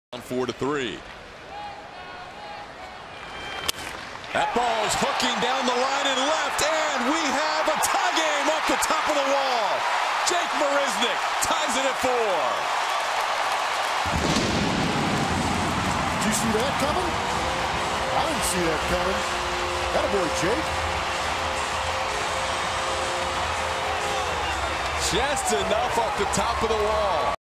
KC@HOU: Marisnick homers, sends it to extras in 9th
Jake Marisnick hits a solo shot off the top of the left-field wall to tie the game at 4 in the bottom of the 9th inning